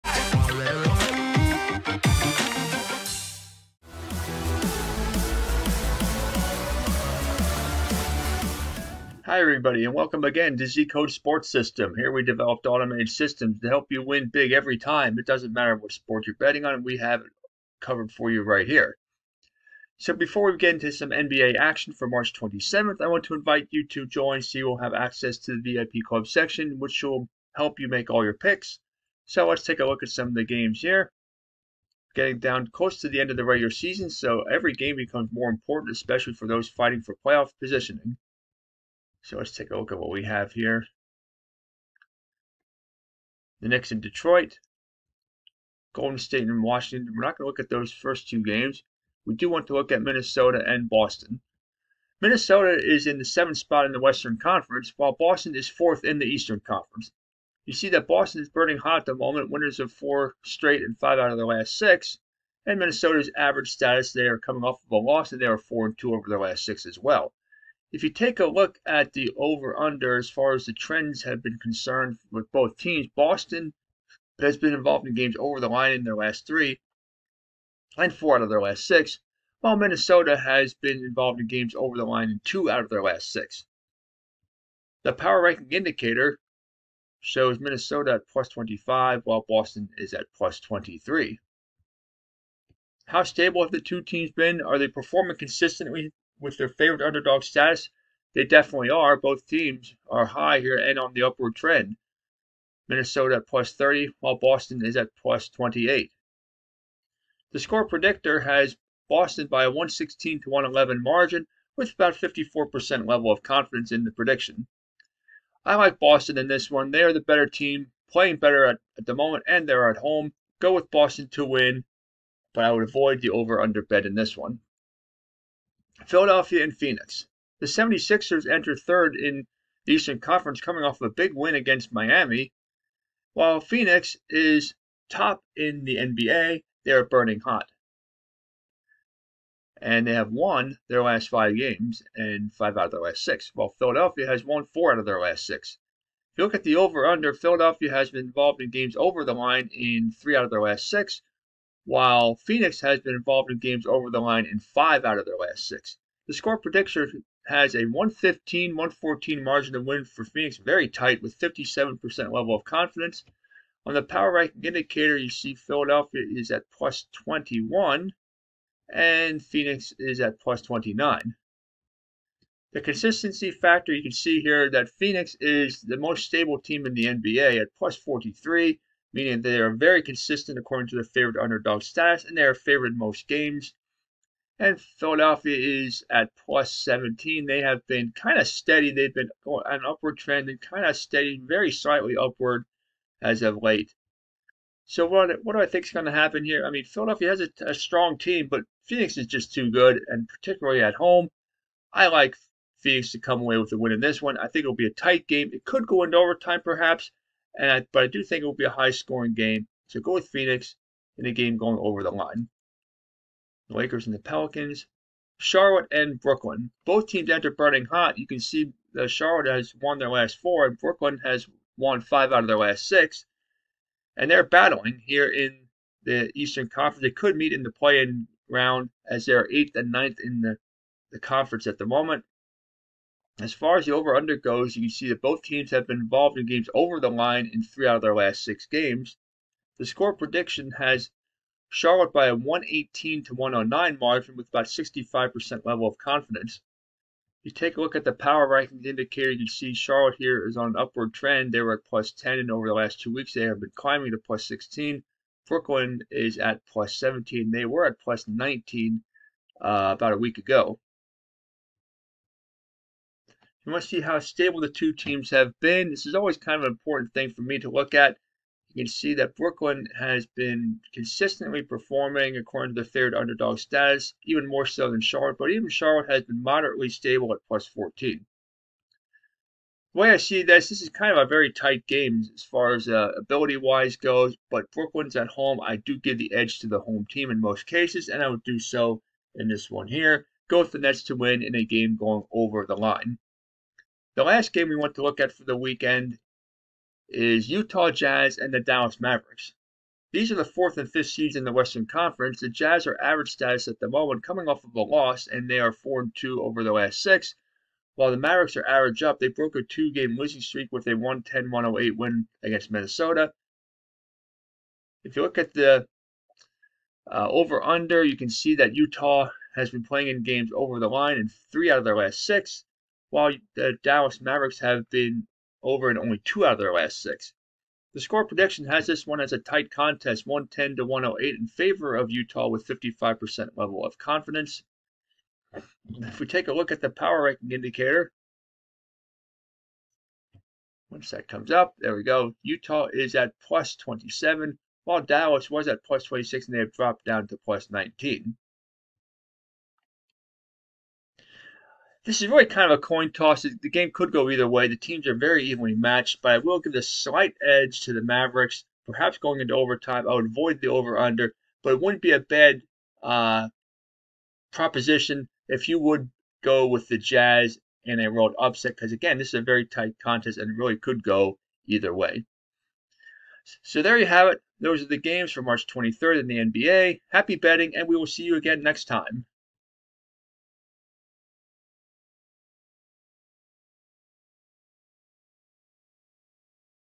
NBA Picks March 27: Philadelphia at Phoenix Headline Action
With the NBA regular season down to the final few weeks, teams are gearing to the postseason. There are several interesting matchups for March 27 with the Suns hosting the 76ers in the featured game.